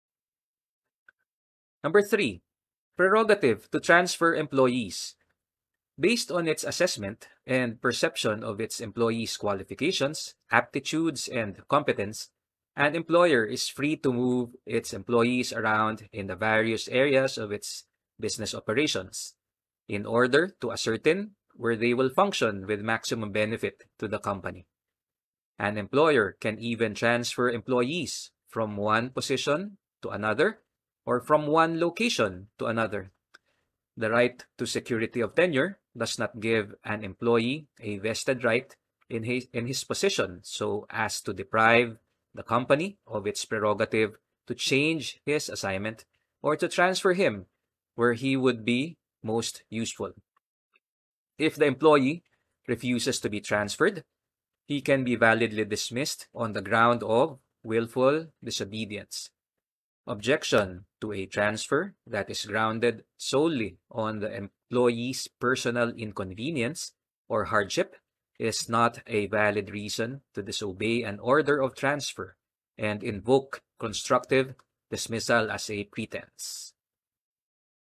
Managerial Prerogatives - Transfer of Employees
Can an employer transfer employees to the various aspects of its business operations? Author Ungos III shares the principles.